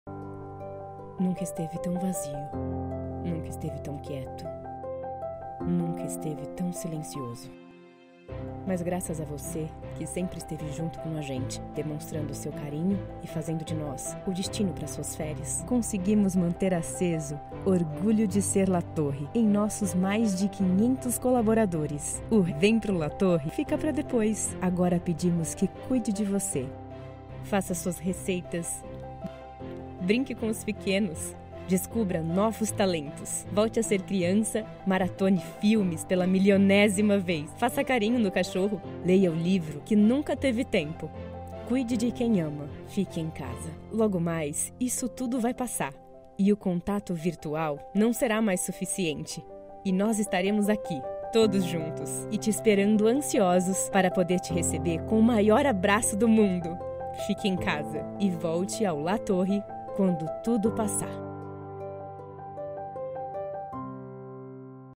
VolteAoLaTorre depois que tudo isso passar
Por agora, vamos ficar em casa e fazer tudo o que adoramos fazer com as pessoas que amamos. Depois volteaolatorre quando tudo passar!
Obrigado a todos os nossos hóspedes que escolheram passar férias conosco. Graças a vocês, nossos colaboradores estão em casa, se cuidando tranquilamente, para quando tudo isso passar, estar de braços abertos a te esperar!